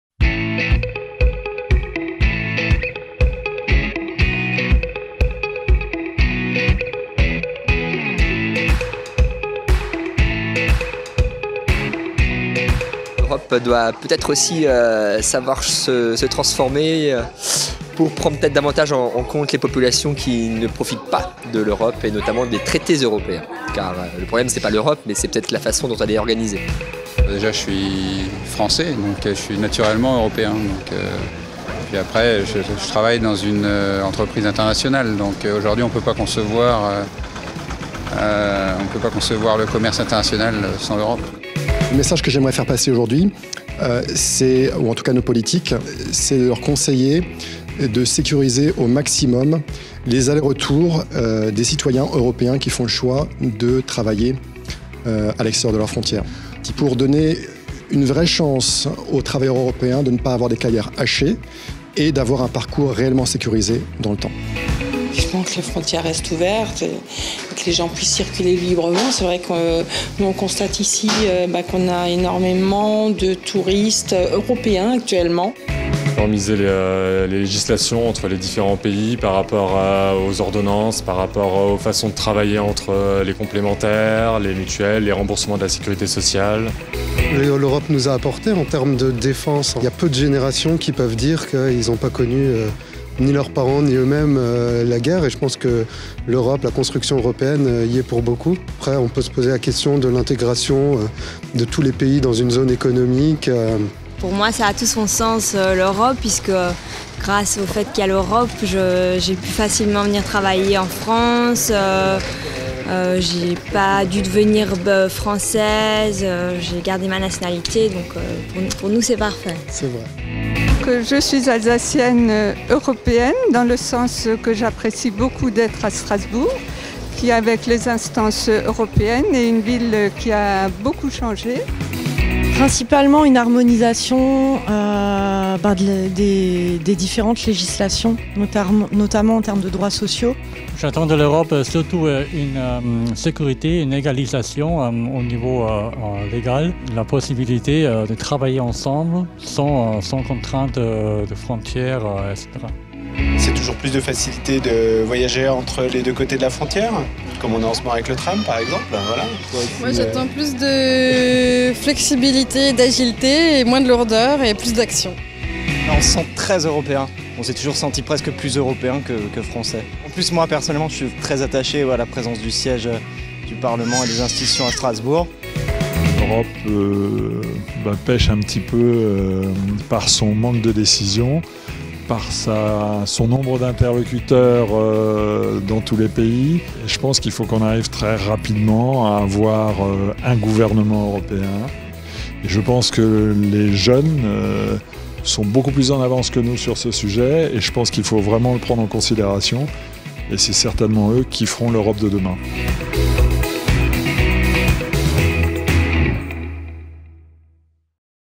La journée de l'Economie Alsacienne 2017
Vidéo d'introduction pour l'événement sous forme de Micro Trottoir.
Organisée par la CCI de Région Alsace, la Chambre de Métiers d’Alsace et la Chambre d’Agriculture de région Alsace, la Journée de l’Economie Alsacienne a eu pour thème en 2017  "Osons l'Europe"